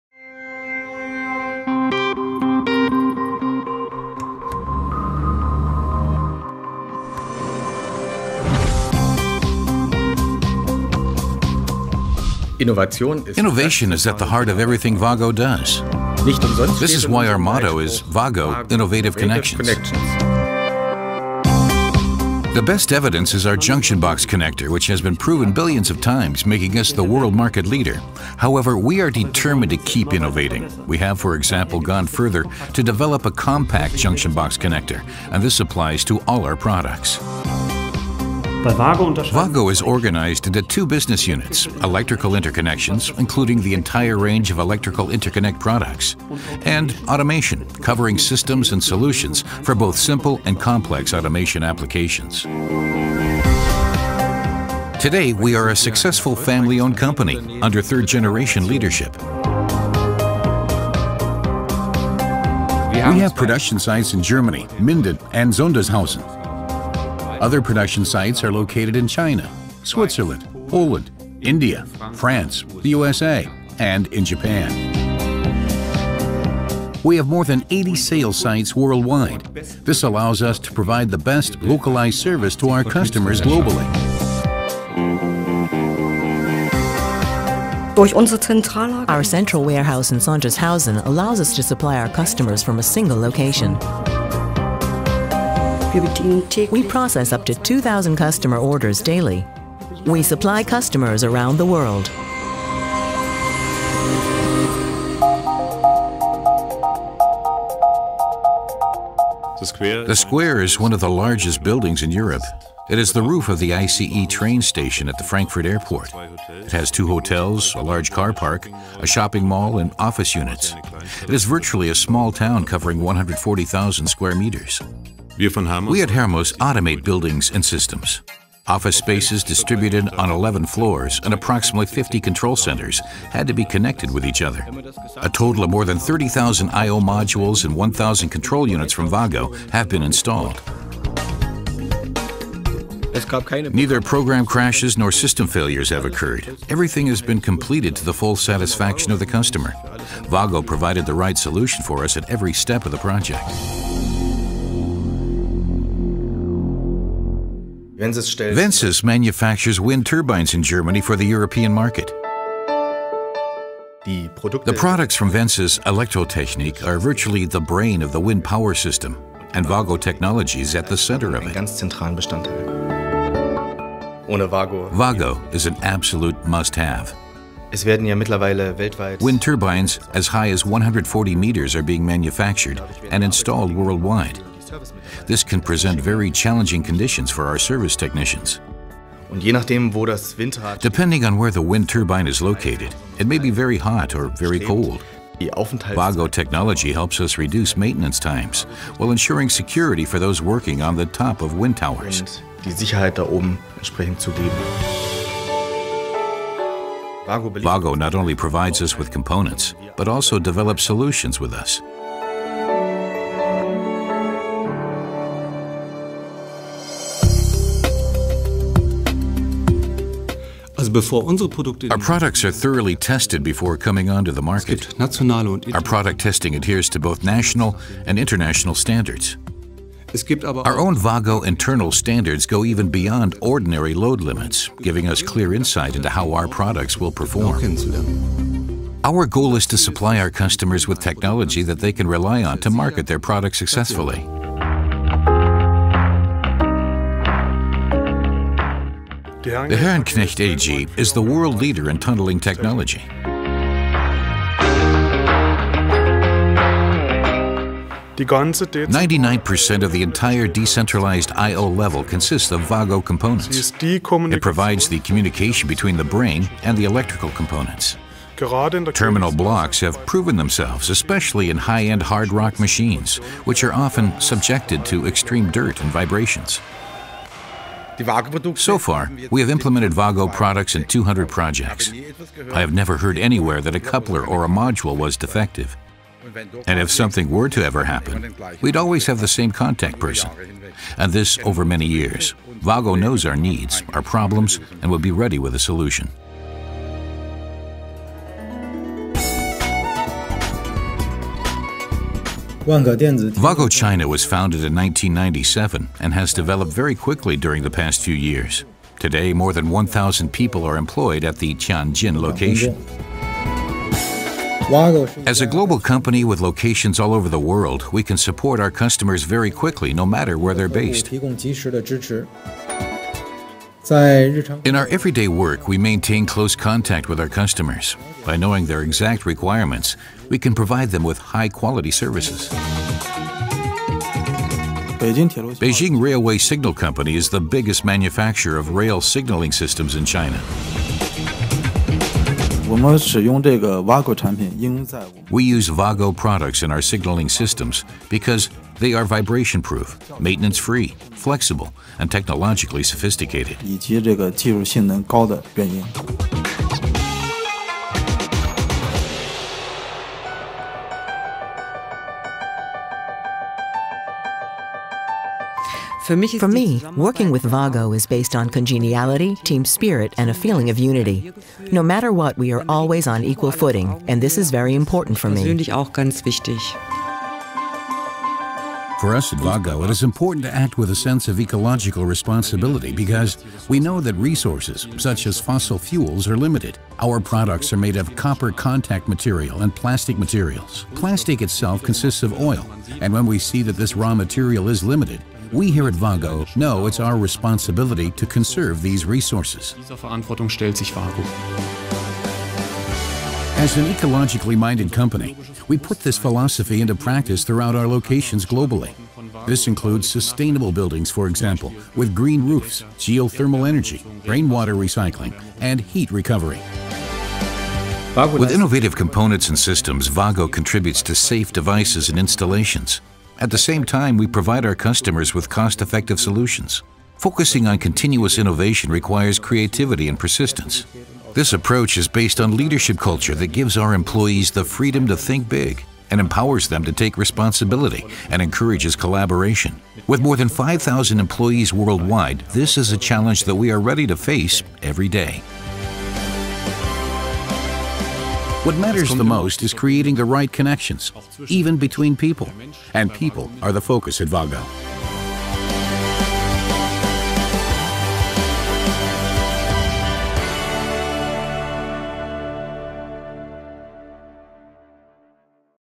Let us introduce ourselves
It began with an idea. To find a better alternative to traditional screw type connection technology. From this idea we developed spring clamp terminal blocks.

WAGO underpinned its conviction that this was the method of the future with the founding of the company in 1951 and has not wavered from this course since then. This technology has long since become the worldwide standard and modern electrical connecting systems and automation without WAGO products is unimaginable.

Many people have contributed to the WAGO success story through their commitment and dedication. All of them believed in the potential of our company and were receptive for new ideas and processes and advanced these ideas with a sincere and passionate effort.